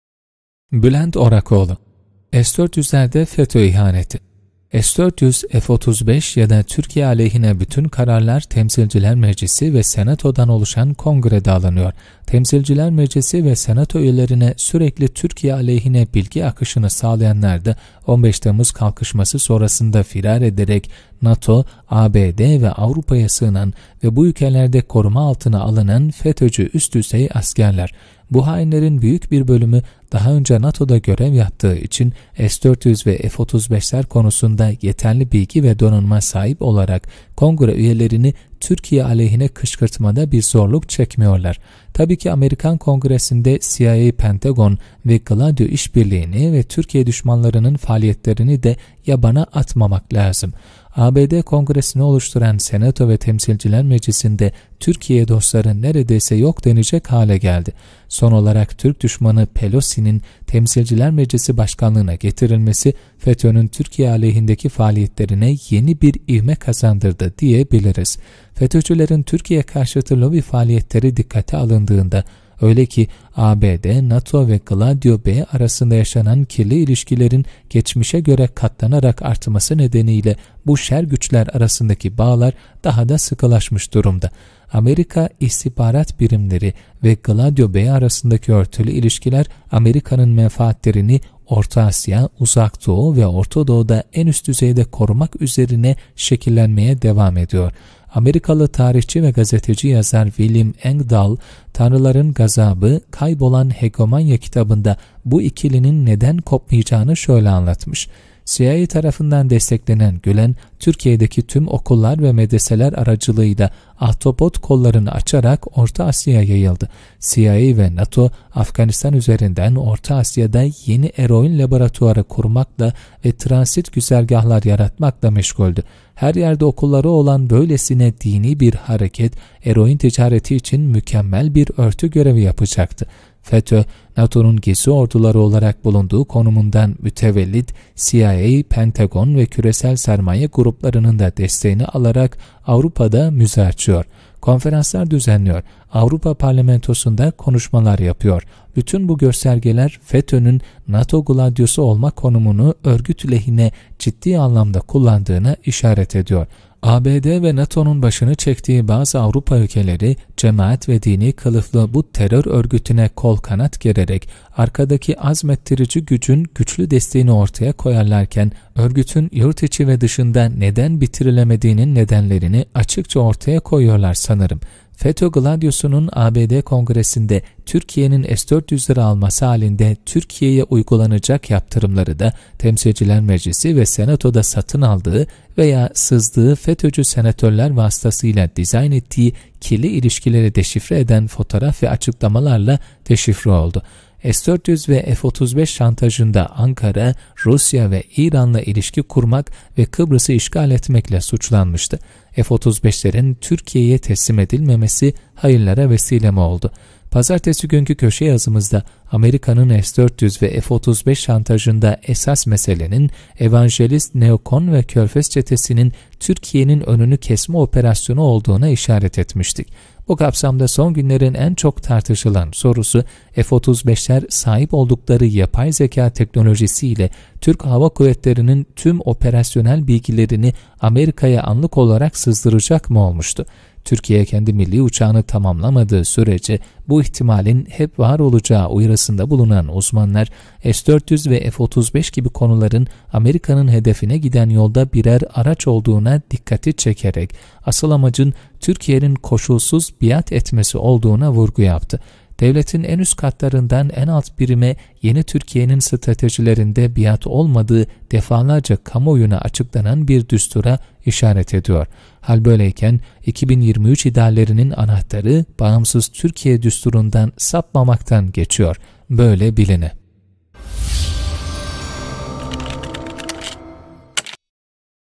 Bülent Orakoğlu - S-400’lerde FETÖ ihaneti - 29.05.2019
S-400, F-35 ya da Türkiye aleyhine bütün kararlar Temsilciler Meclisi ve Senato’dan oluşan Kongre’de alınıyor. Temsilciler Meclisi ve Senato üyelerine sürekli Türkiye aleyhine bilgi akışını sağlayanlar da 15 Temmuz kalkışması sonrasında firar ederek NATO, ABD ve Avrupa’ya sığınan ve bu ülkelerde koruma altına alınan FETÖ’cü üst düzey askerler.